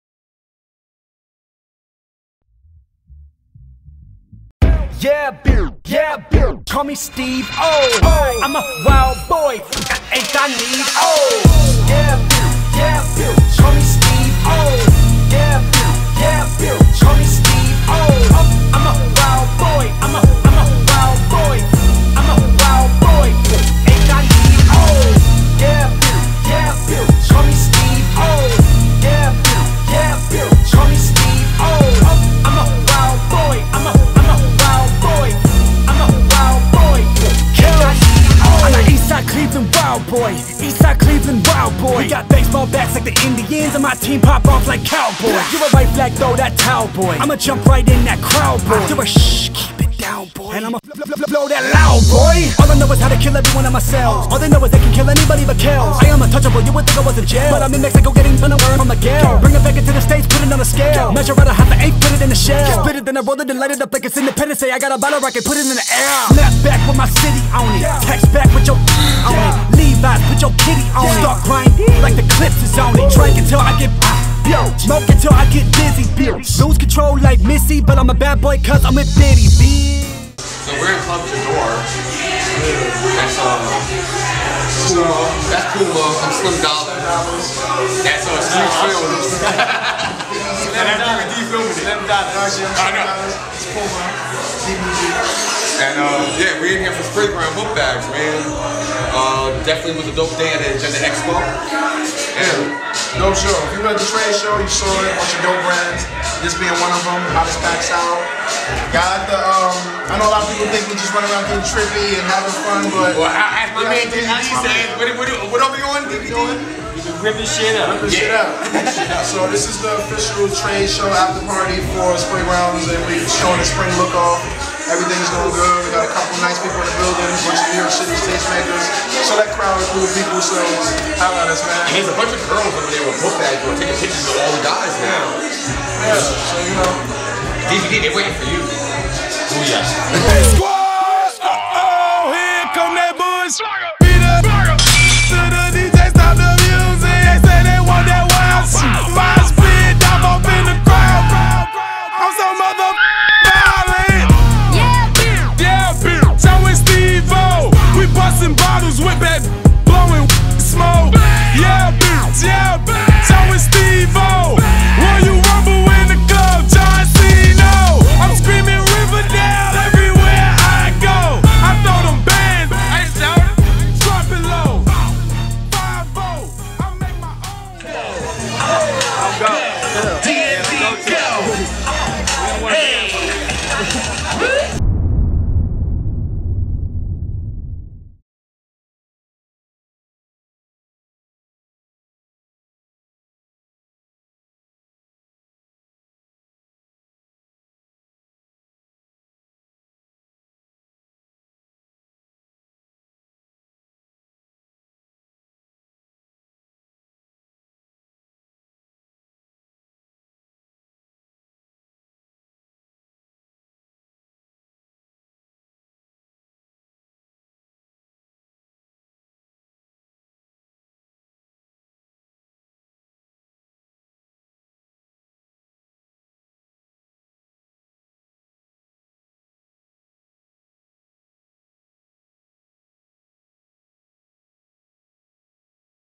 Sprayground Rip Shit Up After Party
DBD Slim Dollars & Puma throw a after party at Club J'Dore Representing Sprayground Book Bags Featuring The (Camo & Ammo Collection) With muisc by DJ Nelly & DJ Supernova